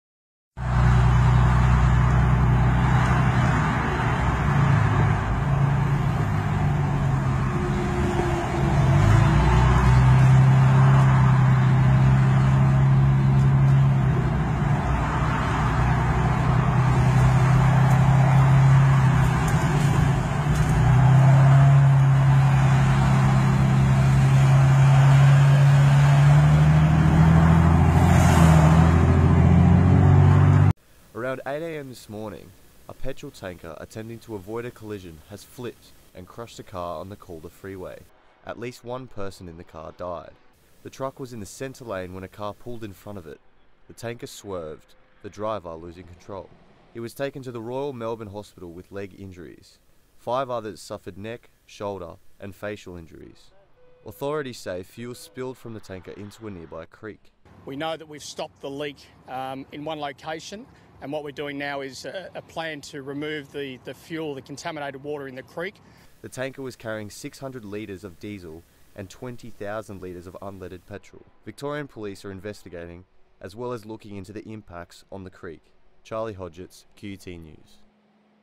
Calder Freeway crash one dead Six injured, Footage before emergency services arrived
Calder Freeway crash one dead Six injured Footage before emergency services arrived, Was  lucky to of left work 10 min late o this  could've been us.